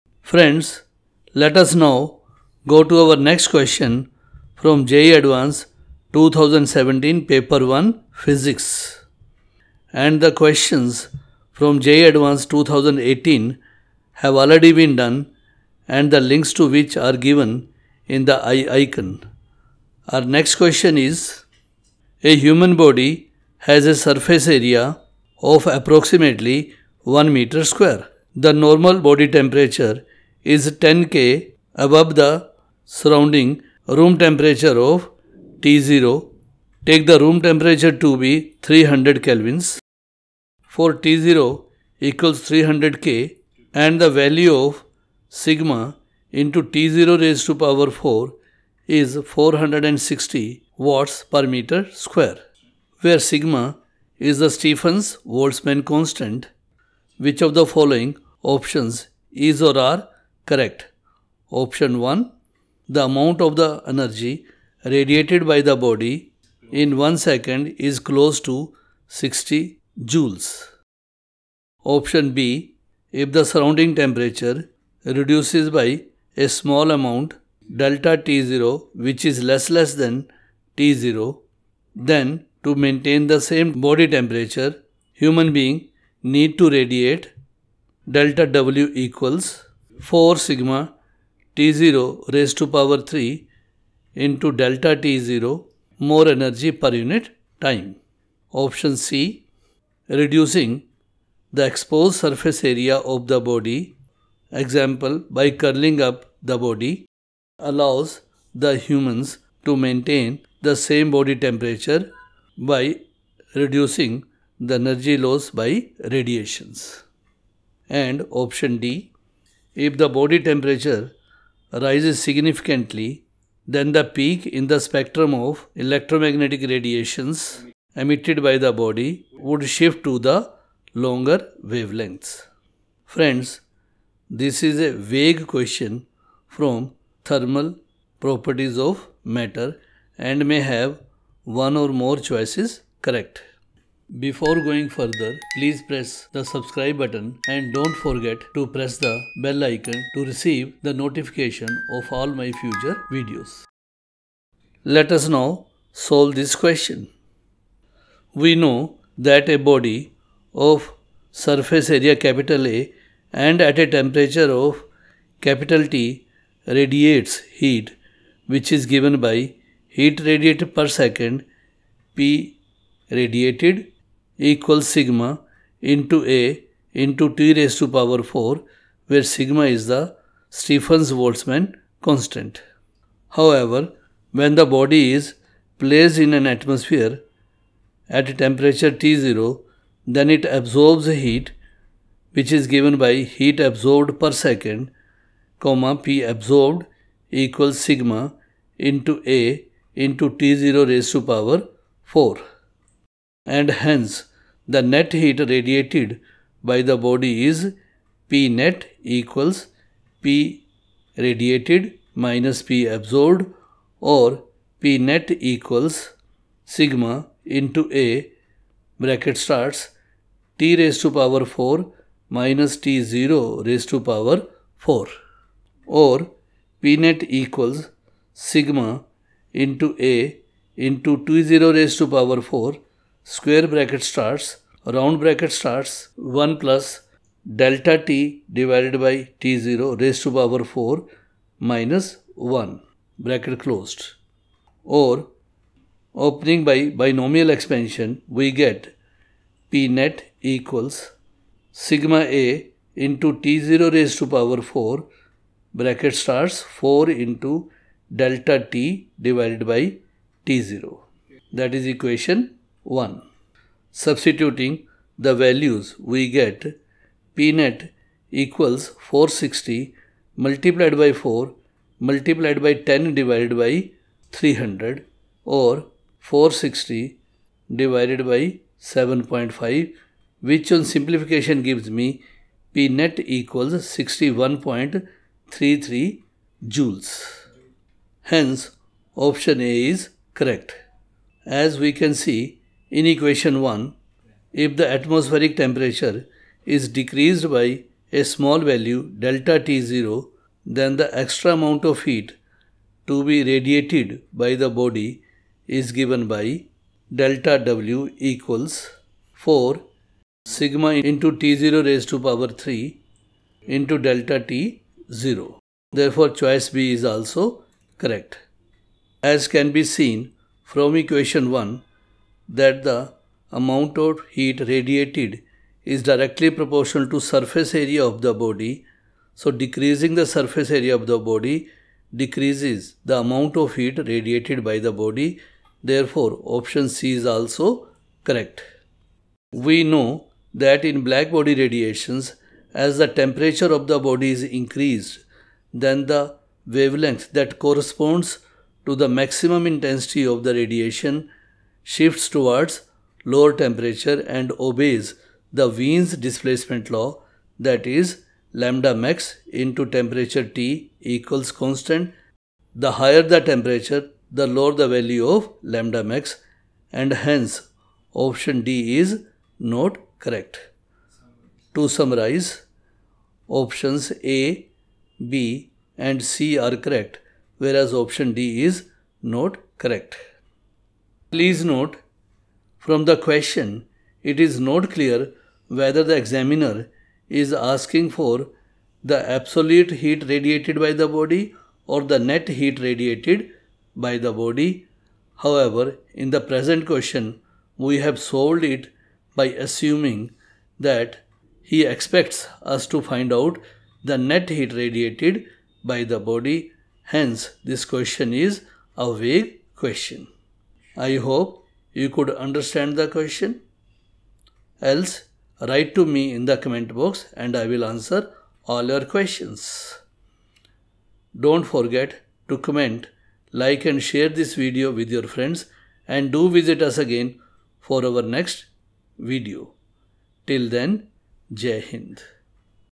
JEE Adv. solutions physics 2017 | A human body has a surface area of approximately 1 m2. The ...
A human body has a surface area of approximately 1 m2. The normal body temperature is 10 K above the surrounding room temperature T0. Take the room temperature to be T0 = 300 K. For To = 300 K, and the value of  T04 =460 Wm-2 (where  is the Stefan-Boltzmann constant). Which of the following option is/are correct?
By Dr. T K Bansal
M.Tech., Ph.D (IIT Delhi)
Postdoctoral fellow University of Oxford
Former Senior Lecturer of Physics IIT Kharagpur
Co-founder of Bansal Tutorials Pvt Ltd